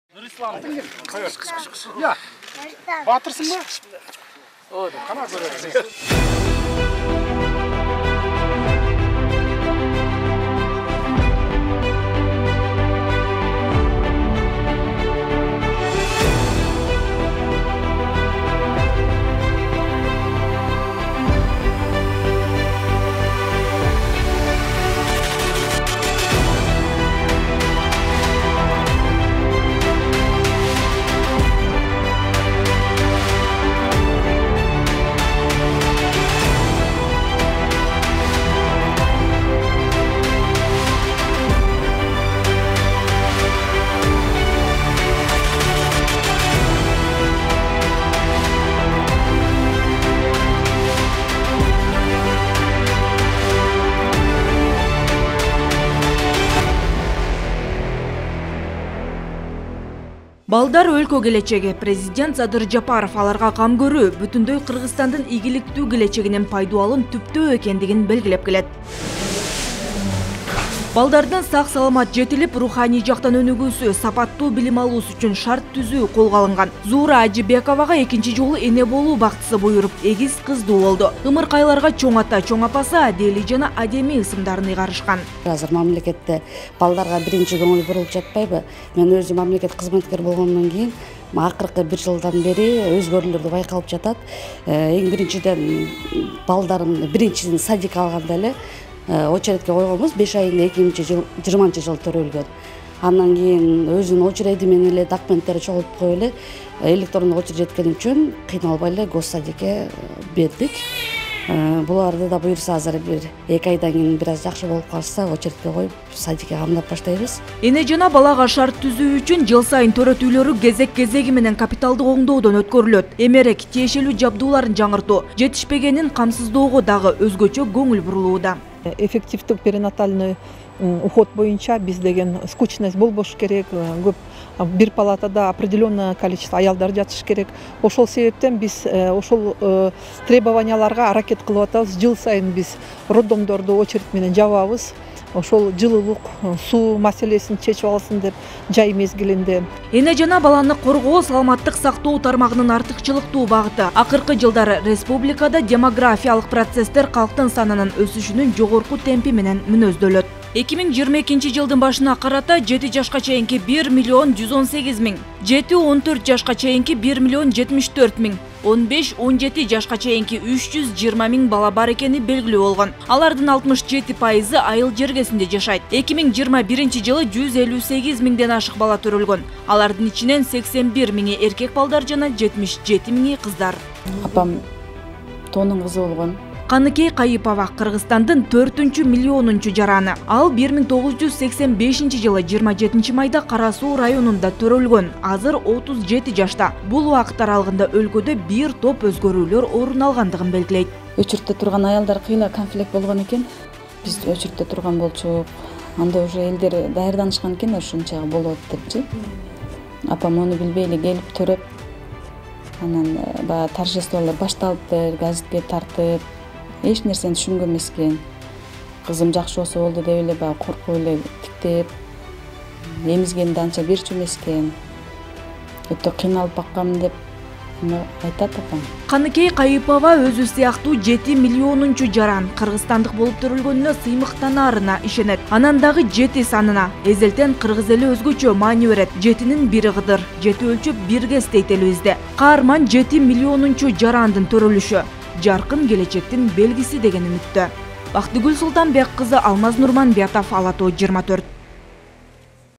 7 миллионунчу кыргызстандык качан төрөлөт?
Өлкөнүн 7 миллионунчу жараны 20-октябрда төрөлөт деп күтүлүүдө. Төрт миллионунчу кыргызстандык менен анын жаш айырмачылыгы туура 37 жыл болот. Бул убакыт аралыгында республика чоң тарыхый өзгөрүүлөрдү башынан өткөрдү. Эң башкысы эгемендүүлүктү туу тутуп, күн сайын өнүгүү жолунда. Президент Садыр Жапаров белгилегендей, өлкөнүн эртеңки келечеги бүгүнкү балдар. Аларга кам көрүү - мамлекеттин социалдык саясатынын маанилүү артыкчылыгы.